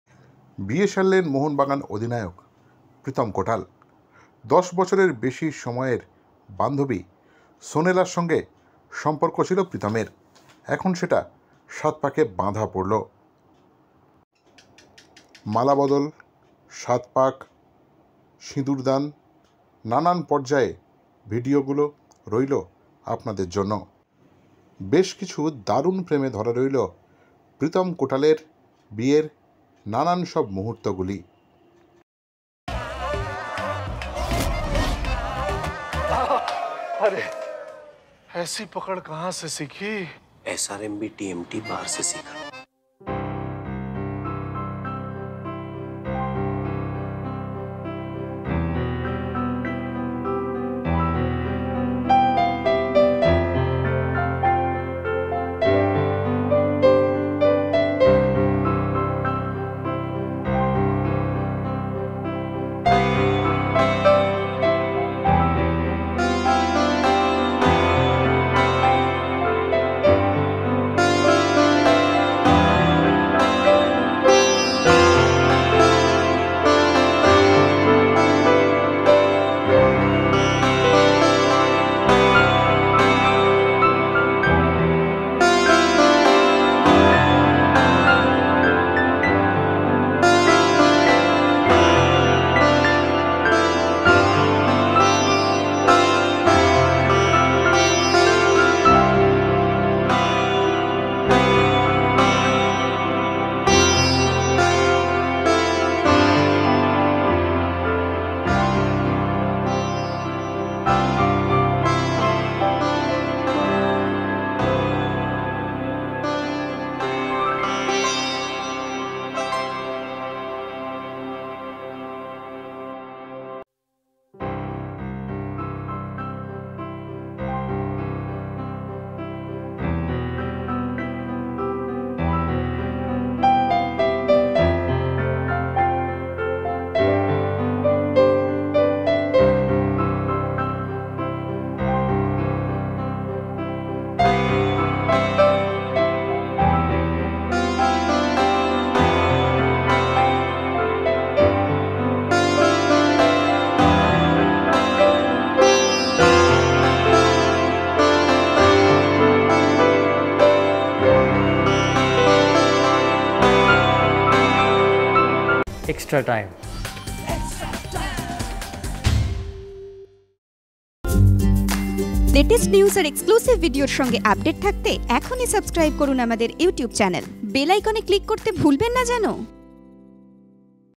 Pritam kotal এর বিয়ে, মালা নিতে চাইছিলেন না সোনেলা! তারপর! সিঁদুর দানের মুহূর্ত। বিয়ের Video
Pritam kotal এর বিয়ে, মালা বদল, সিঁদুর দানের মুহূর্ত। বিয়ের Video
Pritam kotal এর বিয়ে, মালা নিতে চাইছিলেন না সোনেলা! তারপর! সিঁদুর দানের মুহূর্ত।